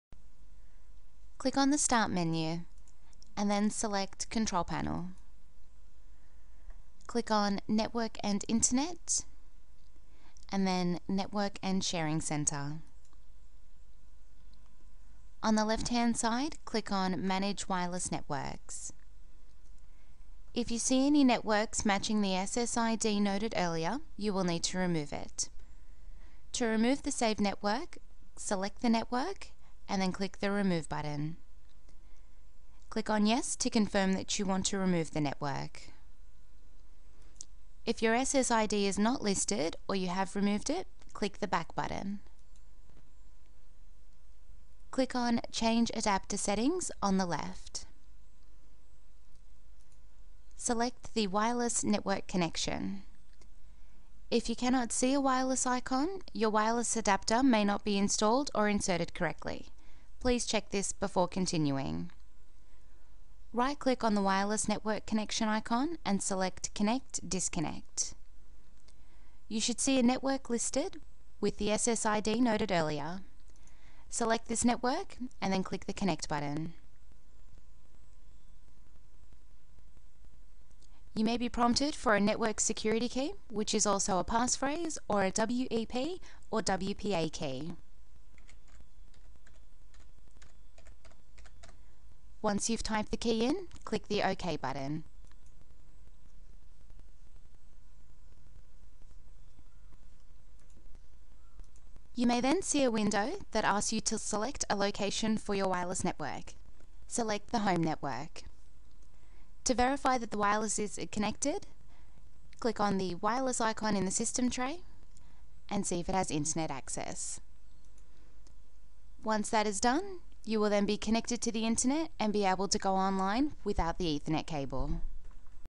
Windows 7 - How to Connect to a Wireless Network
This Video will helps you to setup your Windows 7 computer to connect to your wireless network.